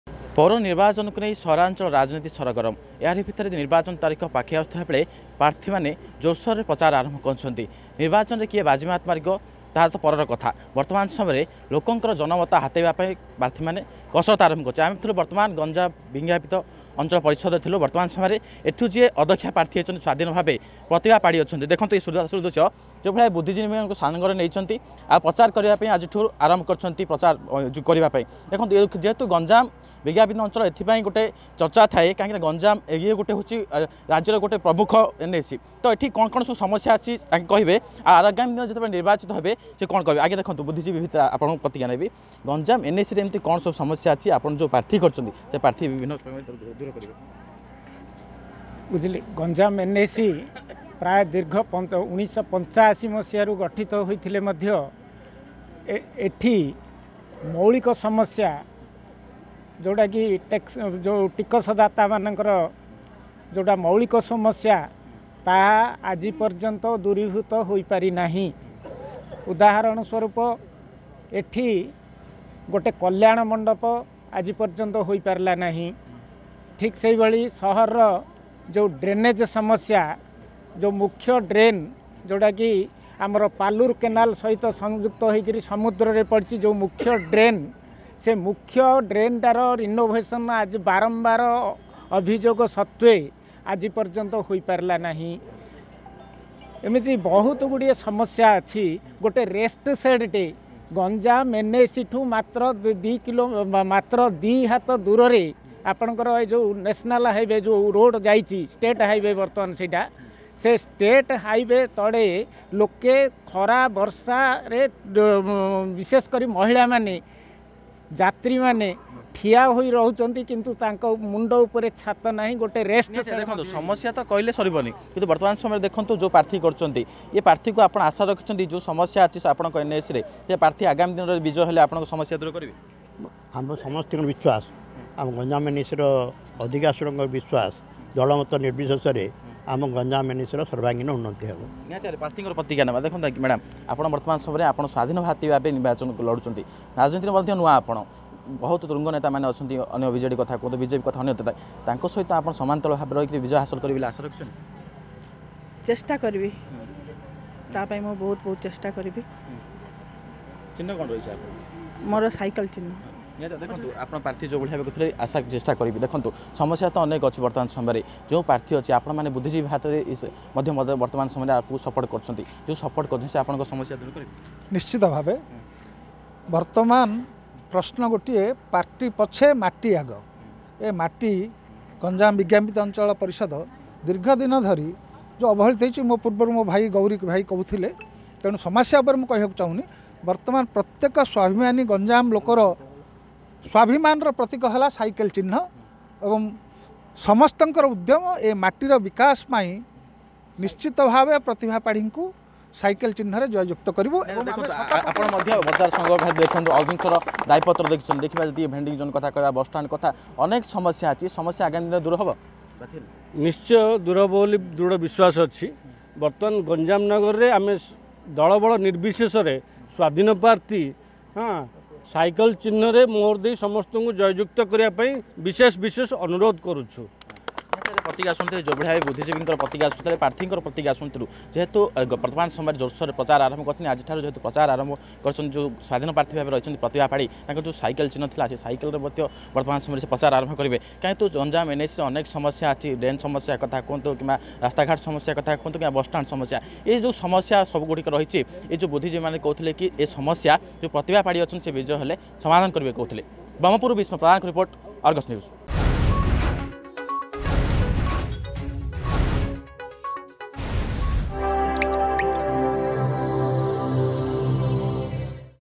Berhampur: Campaigning is in Full swing by Candidates ahead of Municipal Elections 2022
Campaigning is in Full swing by Candidates in Berhampur ahead of Municipal Elections 2022.